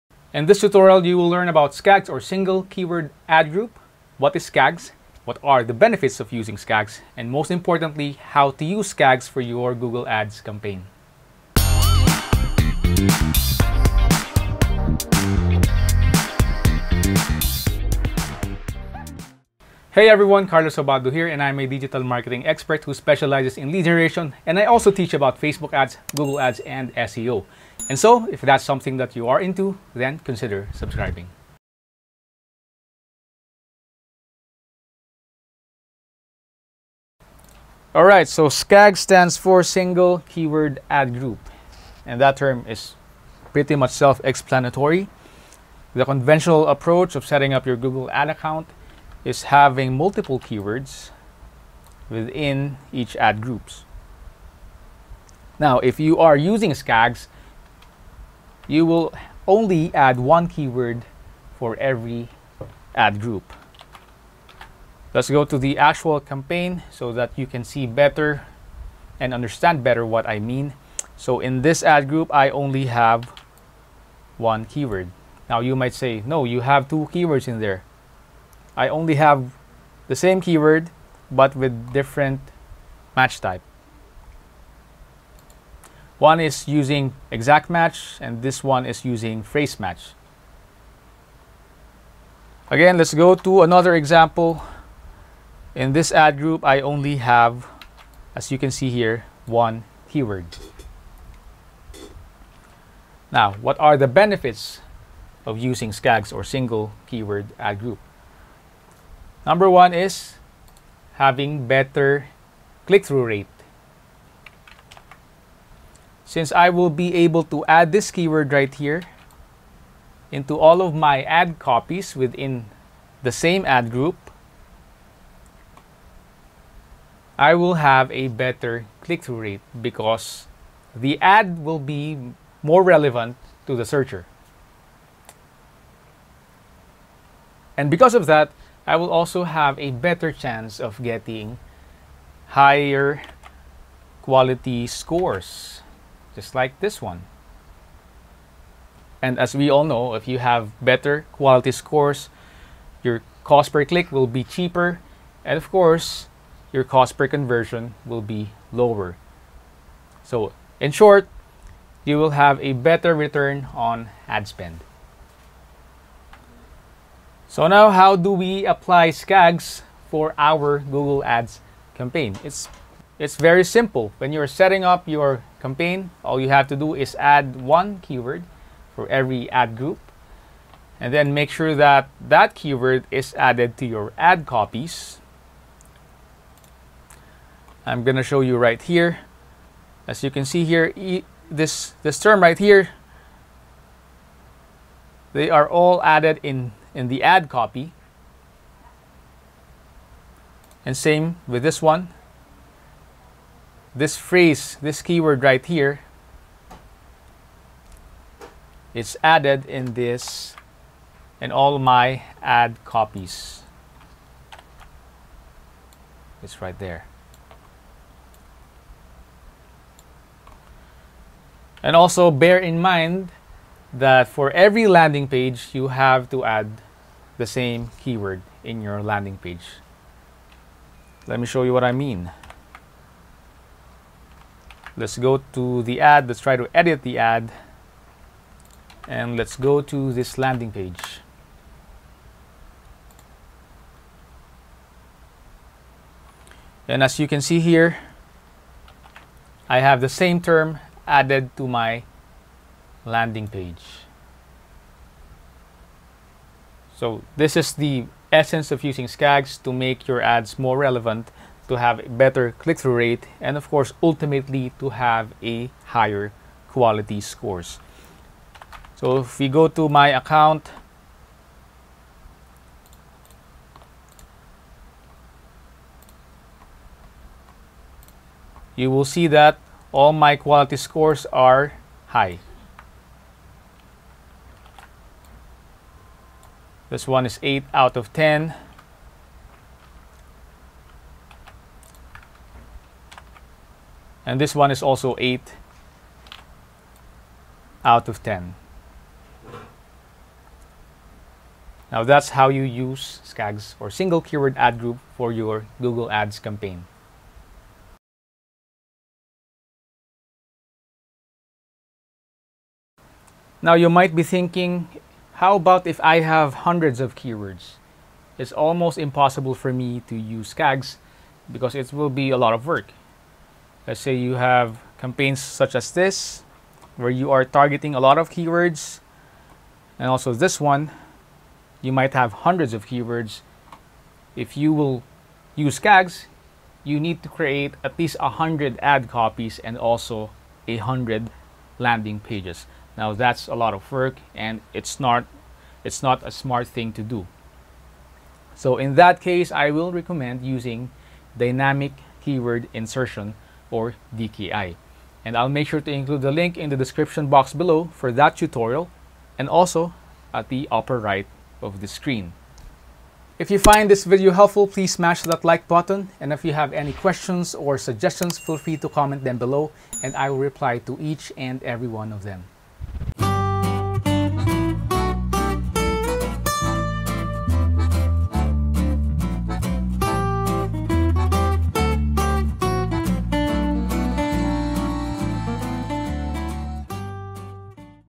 Google Ads Tutorial - SKAGs (Single Keyword Ad Groups)
In this tutorial you will learn what SKAG is, its benefits, and how you can apply it to your Google Ads campaign.

Timestamps
0:00 - Introduction
0:32 - What Is SKAG (Single Keyword Ad Group)
1:42 - What Are the Benefits of SKAG
2:47 - How to Use SKAG
5:07 - Alternative to SKAG (for Bigger Campaigns)